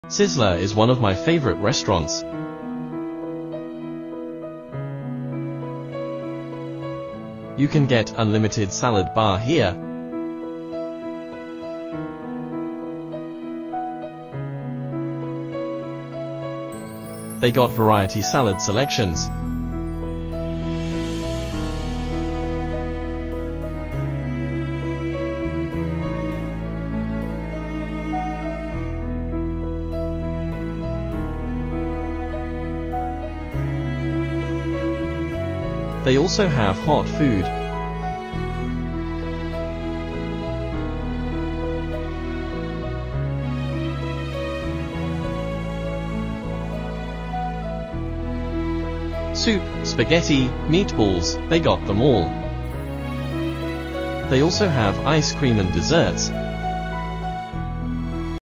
Sizzler Restaurant
Sizzler was a well-known American restaurant chain specializing in casual dining and serving a variety of grilled dishes, salads, and seafood. You can get unlimited salad, and hot food in this restaurant.

I hope you enjoy this video. Simple life is happy life.